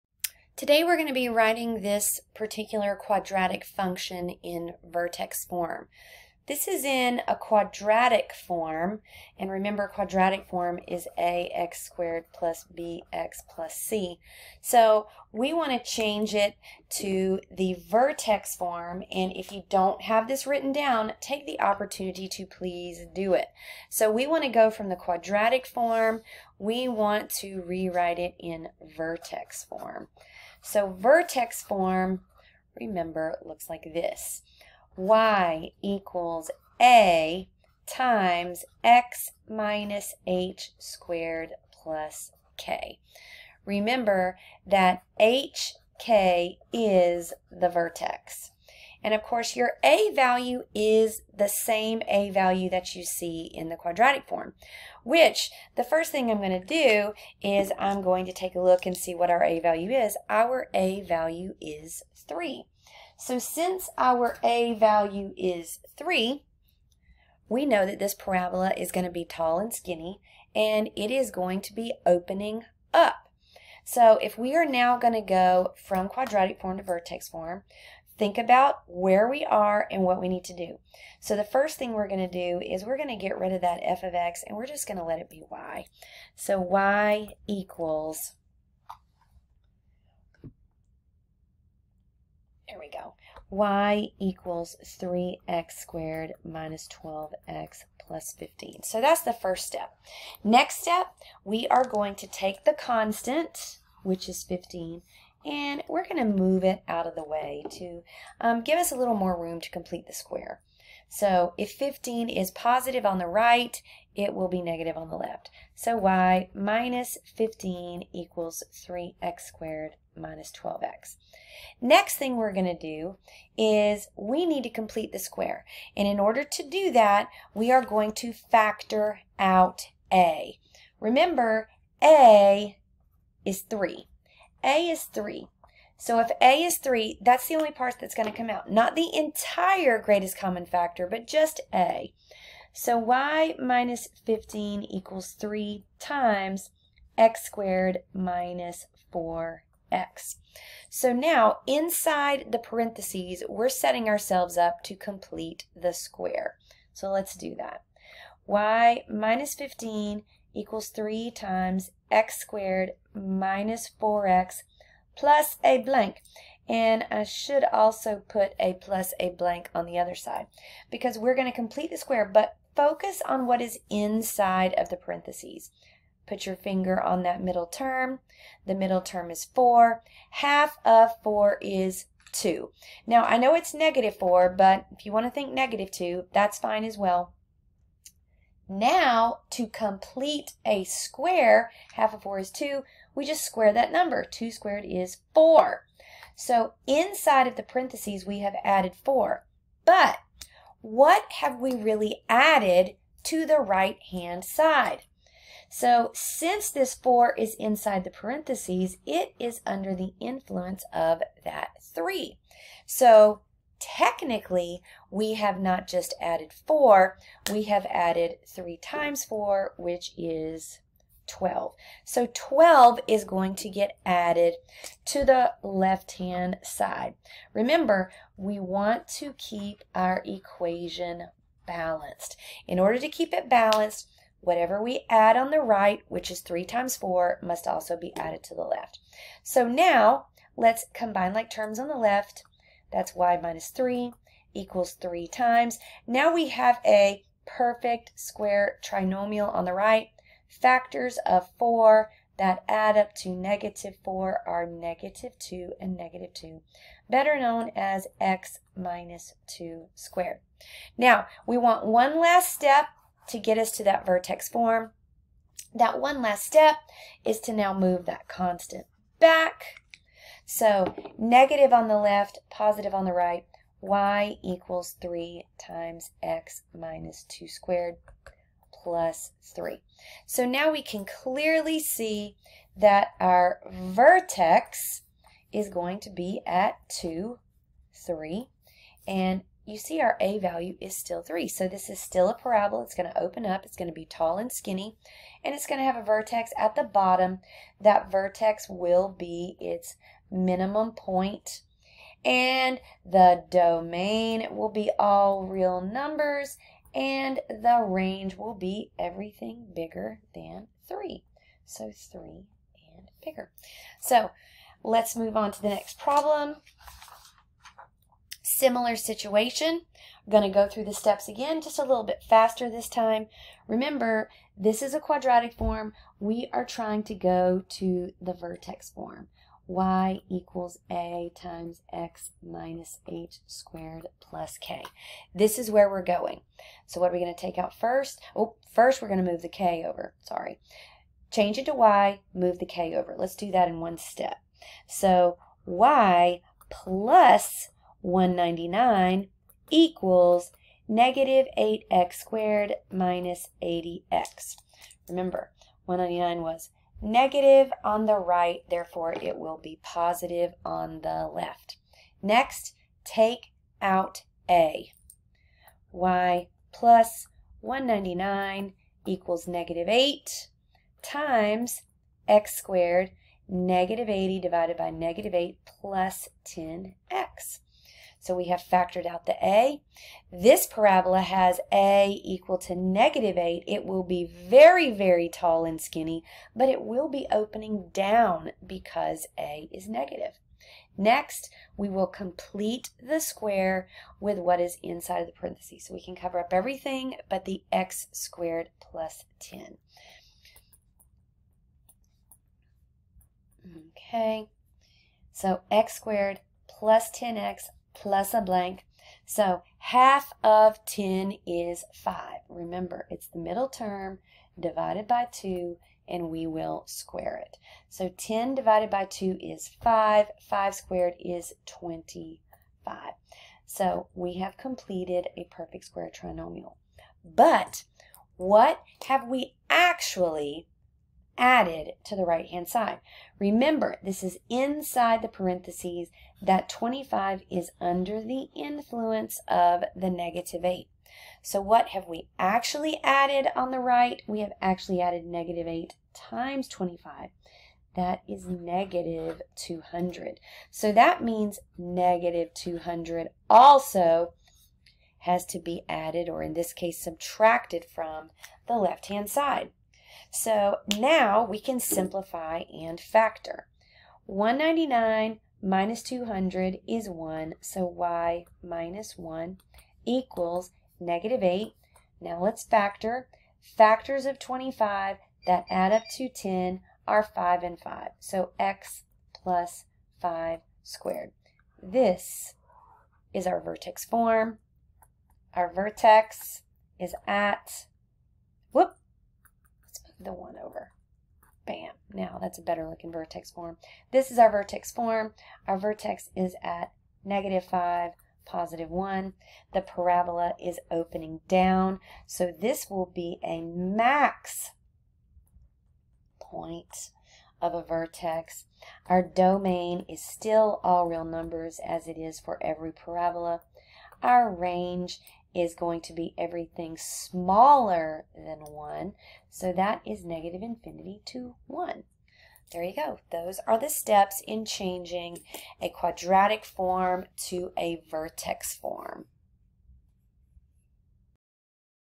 ALG 2 writing a quadratic function in vertex form (h,k)
This video shows you how to change, by completing the square, a quadratic function that is in expanded form to vertex form.